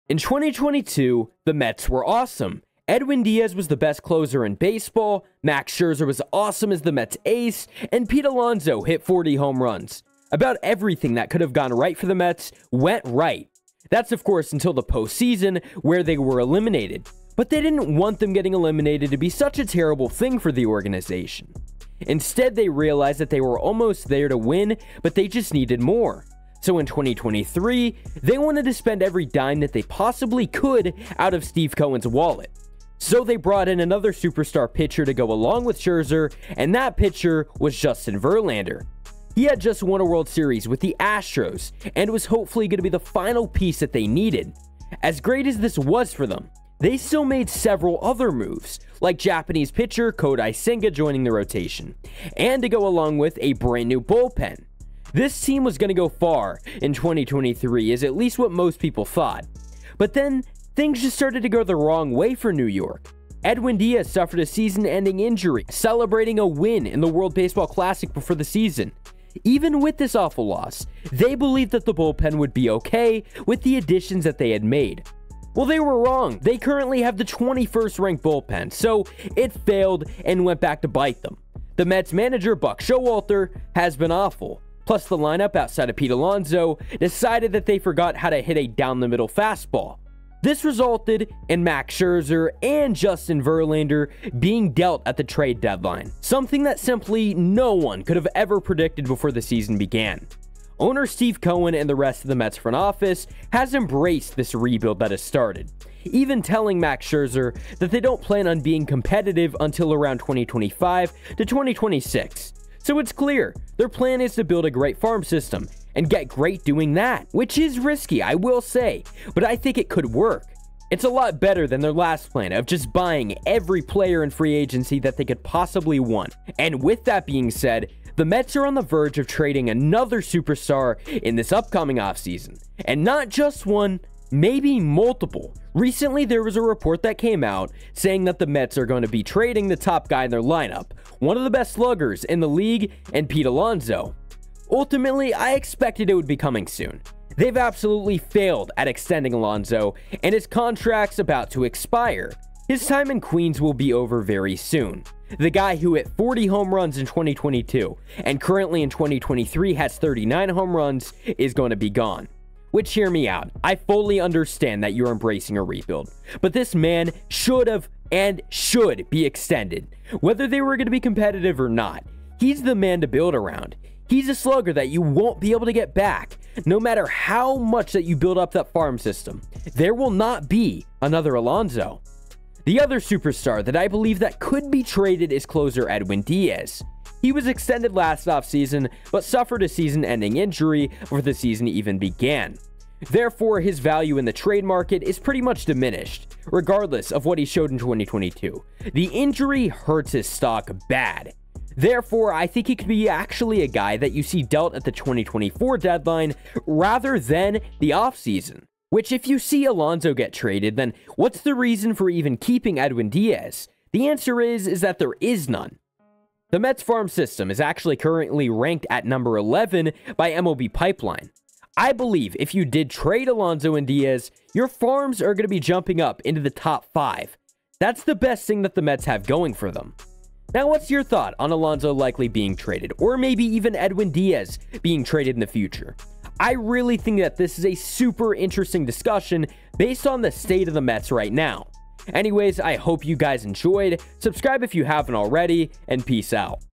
The Mets Are Trading ANOTHER Superstar
In this video, we go over the NEXT Superstar to be traded from the Mets!

Purely Football:  @PurelyFootballYT

The Mets are Trading MULTIPLE Superstars
Title: The Mets Are Trading ANOTHER Superstar
By: Purely Baseball